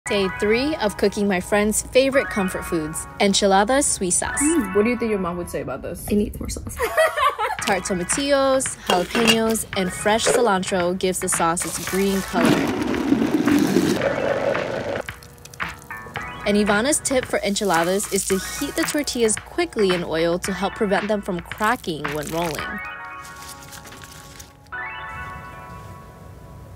Day 3 Of Cooking Comfort Foods From Every Country: Enchiladas Suizas, Mexico
Recipe by @ivskitchen

INGREDIENTS
8 green tomatillos, husked
2 jalapeños
¼ medium yellow onion
4 ounces cream cheese
1 teaspoon chicken bouillon powder
6 garlic cloves
1 cup Mexican crema
½ bunch fresh cilantro, plus more, chopped, for garnish
2 tablespoons olive oil, plus more as needed
15 medium corn tortillas
1 pound shredded rotisserie chicken
8 ounces freshly grated manchego or Monterey Jack cheese
Sliced red onion, for garnish

PREPARATION
Fill a large saucepan halfway with water. Bring to a boil over high heat, then add the tomatillos, jalapeños, and onion and cook for about 10 minutes, or until the tomatillo skins begin to split. Drain, then transfer the vegetables to a blender.
Add the cream cheese, chicken bouillon, garlic, crema, and cilantro bunch to the blender with the vegetables and blend on high speed until smooth, 30–45 seconds.
Add the olive oil to the same large saucepan used to boil the vegetables, then pour in the sauce. Cook over medium heat, stirring occasionally, for about 5 minutes, or until the color turns a shade paler.
Heat a drizzle of olive oil in a large skillet over low heat. Working in batches, add the tortillas and cook for 20–30 seconds on each side to soften, adding more oil between batches if needed.
In a medium bowl, pour ¾ cup of the enchilada sauce over the shredded chicken and toss well until the chicken is fully coated.
Add ½ cup of the enchilada sauce to a 9 x 13-inch baking dish and smooth with an offset spatula until the entire bottom of the dish is covered.
Add some shredded chicken to each tortilla, roll up, and arrange seam-side down in the baking dish on top of the enchilada sauce. Evenly pour the remaining sauce over the rolled tortillas, then sprinkle the grated cheese on top.
Bake the enchiladas for 30–40 minutes, or until the cheese melts and begins to turn golden.
Garnish the enchiladas with sliced red onion and chopped cilantro, then serve immediately.